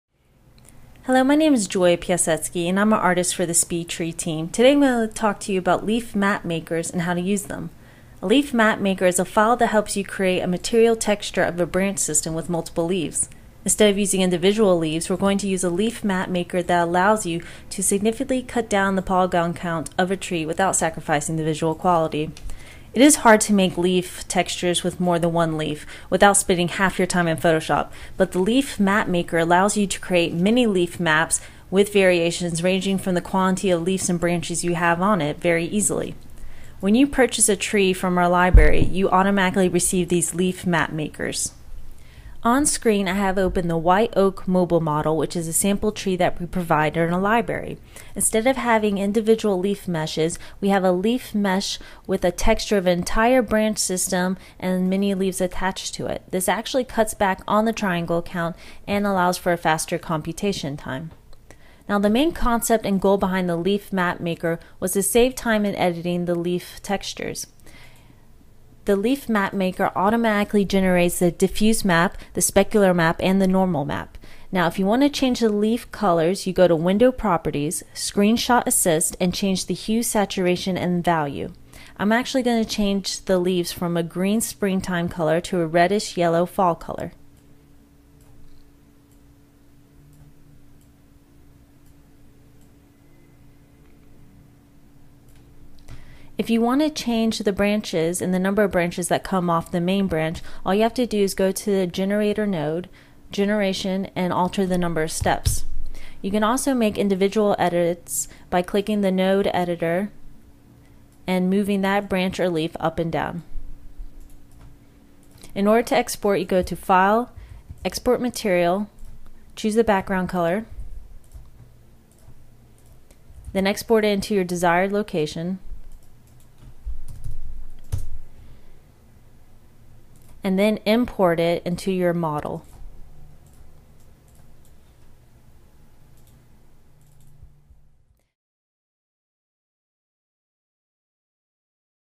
SpeedTree Leaf Map Makers Explained
For tree models where it’s impractical to model every leaf separately, creating convincing leaf clusters is important (example at right). Many of our models include SPM files to help generate these automatically. Using the Modeler, users can easily tweak the individual leaf sizes, quantities, and color variations. The Modeler will also automatically generate the diffuse, normal, specular and transmission masks – all without ever opening Photoshop®.